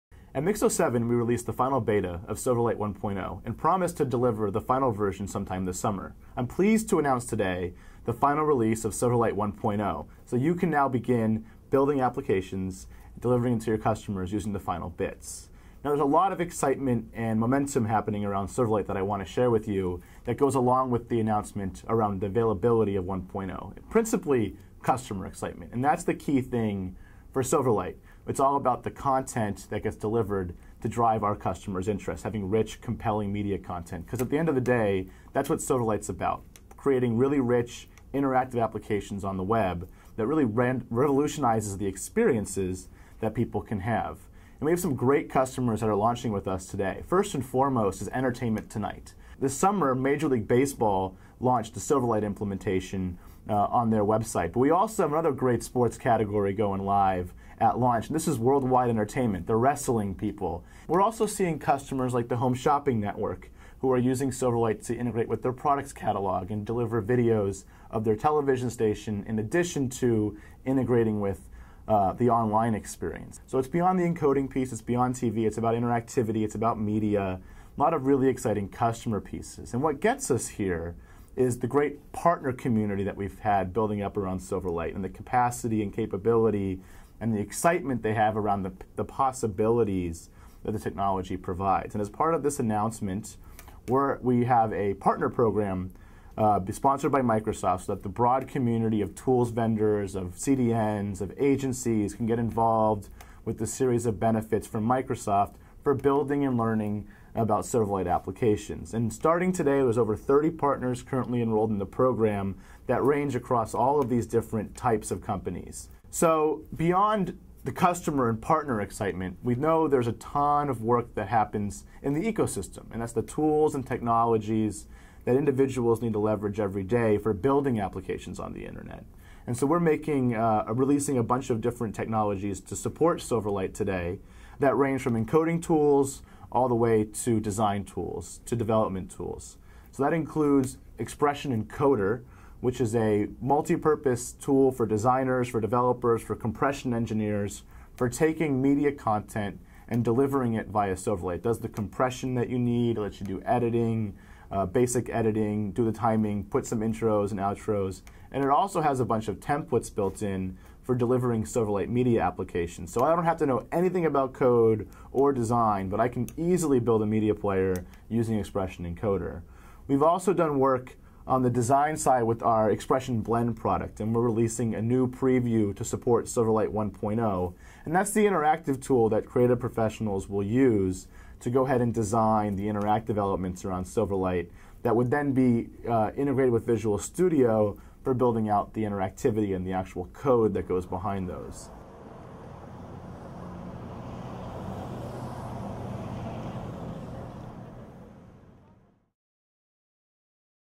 Brian Goldfarb Silverlight Announcement
Breaking: Microsoft Makes "Flashy" Hollywood Debut with Silverlight. and Extends Support to Linux

Microsoft just released Silverlight 1.0, the cross-platform, cross-browser plug-in for streaming video, games and other multi-media content. The company has a number of content partners who will use Silverlight including Entertainment Tonight, HSN, World Wrestling Entertainment.

Entertainment Tonight launches today in Silverlight.  (As reported on Beet.TV Major League Baseball launched in an earlier version of Silverlight in late July.)

Like Flash, Silverlight is a thin plug-in download.

It works on all major browsers and operating systems including Linux.  Microsoft is working with Novell to deliver Silverlight support for Linux, called Moonlight.  This has been much speculation about this development.

We spoke with Brian Goldfarb who heads up development for Silverlight.   Brian talks about the platform, its launch and the design tools that are being released.

Beet.TV was on location at Microsoft's headquarters in Redmond last week where our intrepid video producer David Kavanaugh interviewed several executives and engineers.  We are editing a number of segments about developments at Microsoft.

Thanks to everyone at Microsoft for such warm hospitality for the purple channel.

Grab the (Flash) embed code for this interview with Brian.

-- Andy Plesser

Posted on Wednesday, September 05, 2007